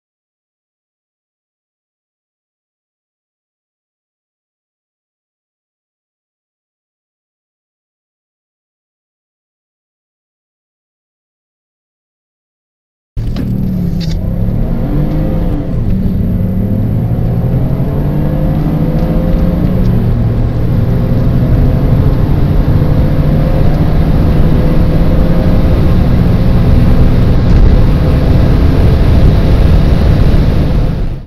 Versus Car Battle: Saab 9-3 2.8T vs Alfa Romeo 159 3.2 JTS V6 Q4
MPL Versus Car Battle
Saab 9-3 Sport Sedan 2.8 Turbo V6
2.8 v6 Turbo 255hp 350nm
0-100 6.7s

Alfa Romeo 159 3.2 JTS V6 Q4
3.2 v6 250hp 322nm
0-100 7s